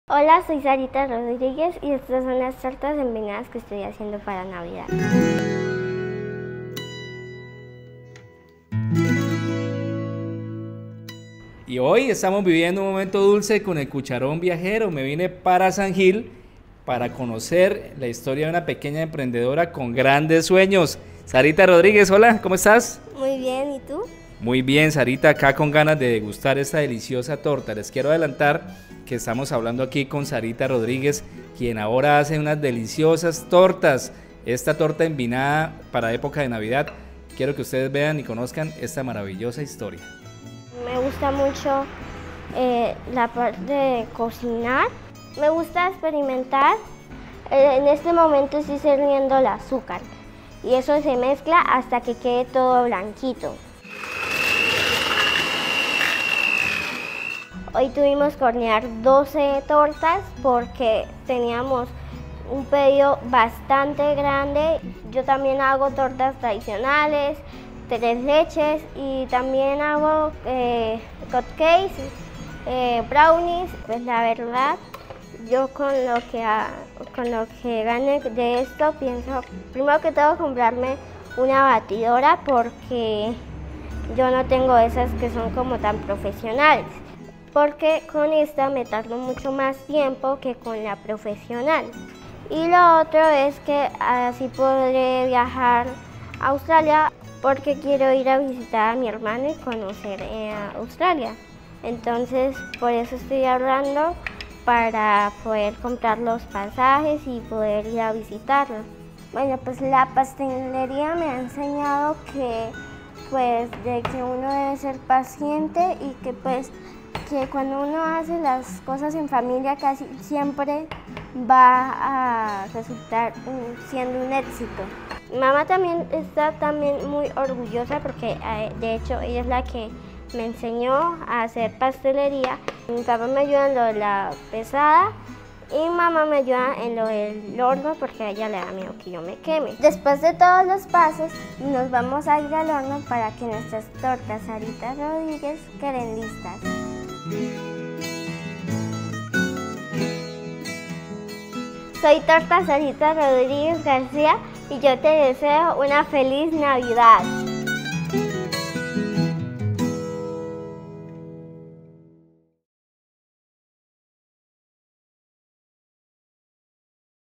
"Niña emprendedora" TORTAS SARITA RODRÍGUEZ
"Pequeños emprendedores con grandes sueños"
En el municipio de San Gil Santander, tuve ¡Un dulce encuentro!
Sarita Rodríguez es una niña de 11 años que se ha convertido en una emprendedora de la pastelería desde hace 2 años cuando descubrió que mezclando sabores podía darle forma a su primer negocio.
Los invito a llenarse de inspiración con esta historia que nos enseña que con alegría y esfuerzo se pueden cumplir las metas.
Importante: Sarita está ahorrando para comprarse una súper batidora profesional para que le rinda más en la preparación de sus tortas y también desea viajar a Australia a visitar a su hermano mayor.
Dónde pedir las toras envinadas para este diciembre?